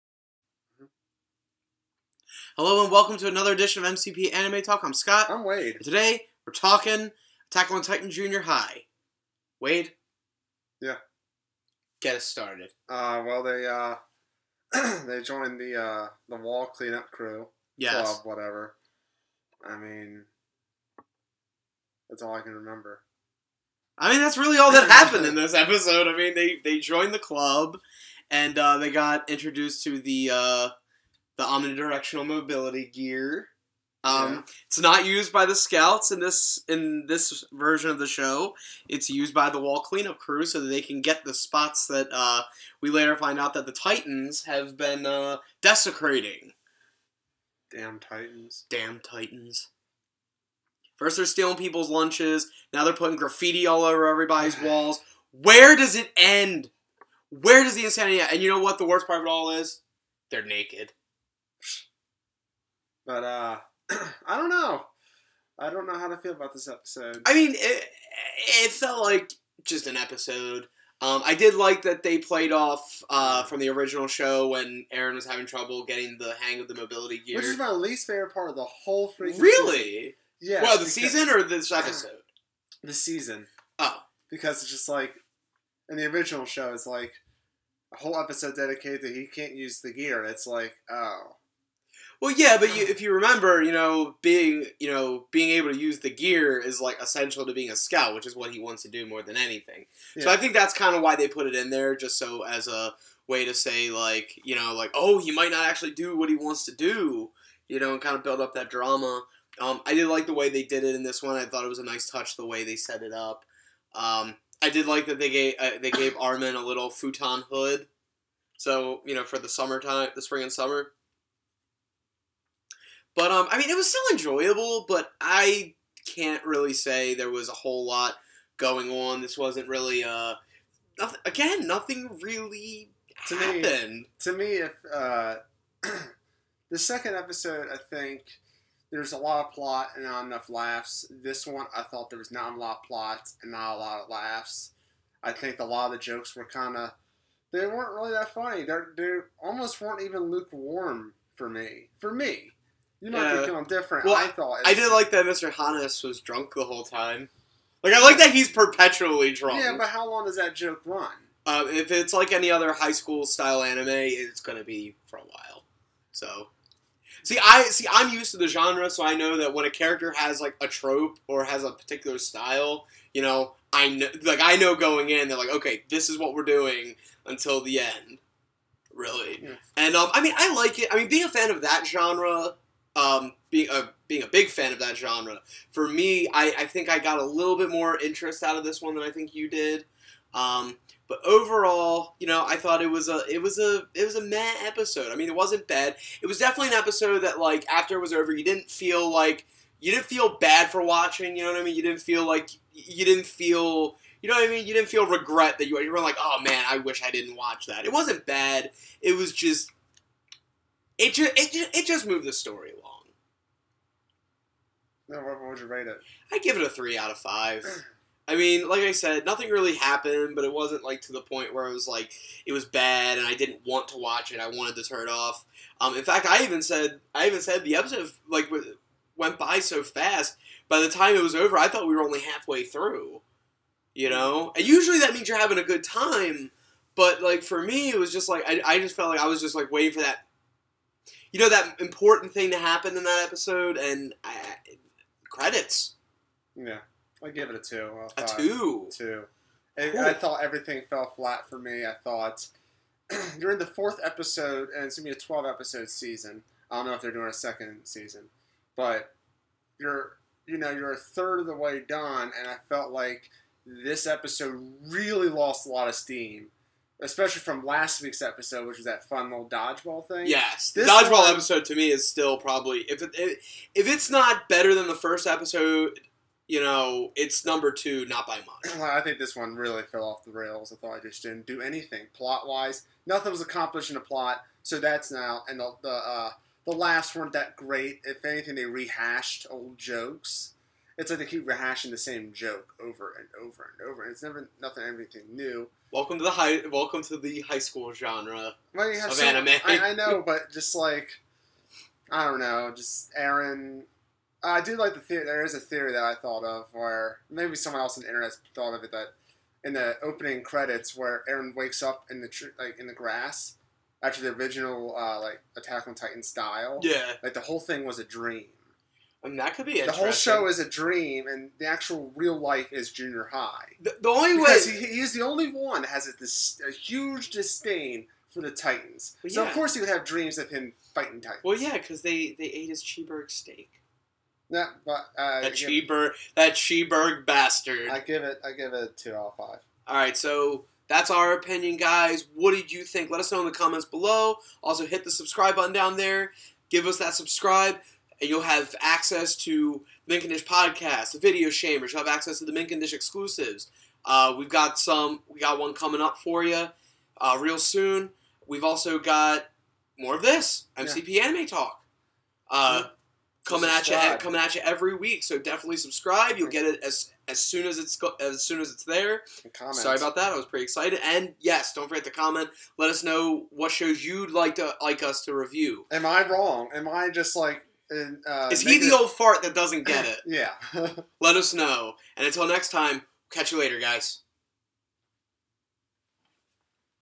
Attack on Titan: Junior High Episode 4 Anime Review
MCP Anime Talk's review of the episode 4 of Attack on Titan: Junior High!! Review may contain some SPOILERS.

Listen to the Mint Condish Podcast every Monday on iTunes and Podbean!
The sources we watch our anime are Hulu, Crunchyroll and Funimation